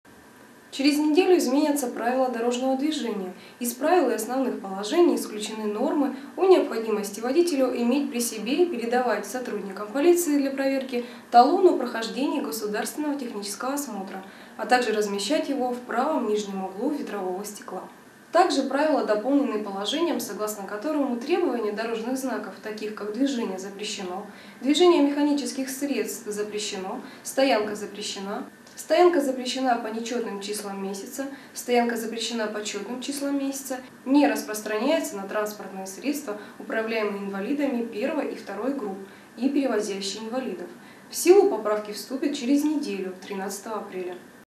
Новости Хакасии: Изменились ПДД
Новое Цифровое Телевидение: Через неделю изменятся правила дорожного движения.
Из Правил и Основных положений исключены нормы о необходимости водителю иметь при себе и передавать сотрудникам полиции для проверки талон о прохождении государственного технического осмотра, а также размещать его в правом нижнем углу ветрового стекла.
Также правила дополнены положением, согласно которому требования дорожных знаков Таких как «Движение запрещено», «Движение механических транспортных средств запрещено»,  «Стоянка запрещена»,  «Стоянка запрещена по нечетным числам месяца»,  «Стоянка запрещена по четным числам месяца» не распространяются  на транспортные средства, управляемые инвалидами I и II групп и перевозящие инвалидов.
В силу поправки вступят через неделю, 13 апреля.